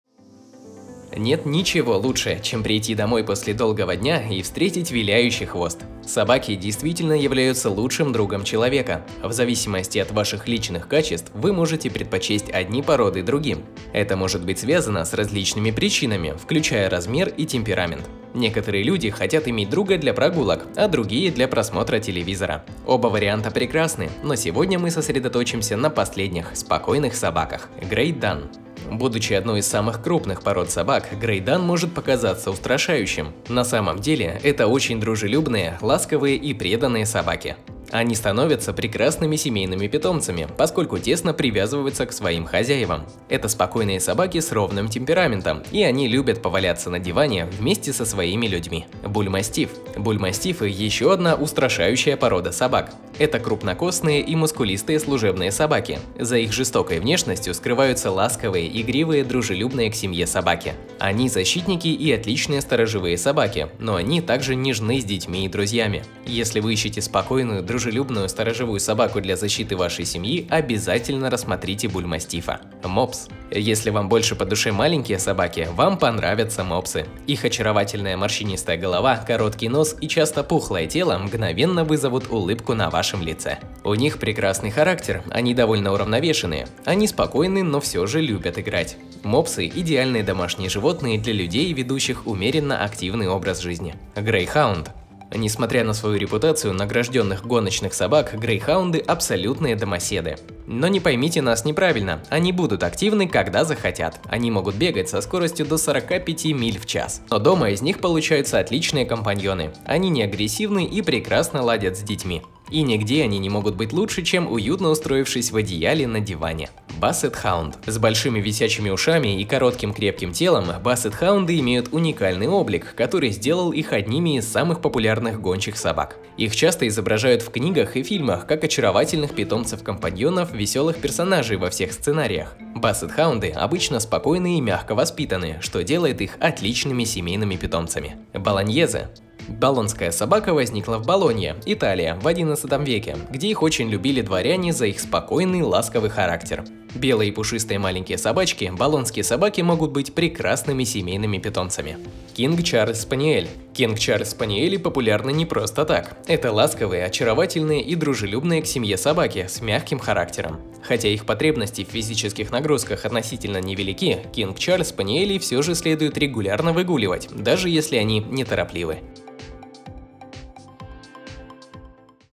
Спокойные породы собак
Нет ничего лучше, чем прийти домой после долгого дня и встретить виляющий хвост. Собаки действительно являются лучшим другом человека. В зависимости от ваших личных качеств, вы можете предпочесть одни породы другим.

Это может быть связано с различными причинами, включая размер и темперамент. Некоторые люди хотят иметь друга для прогулок, а другие - для просмотра телевизора. Оба варианта прекрасны.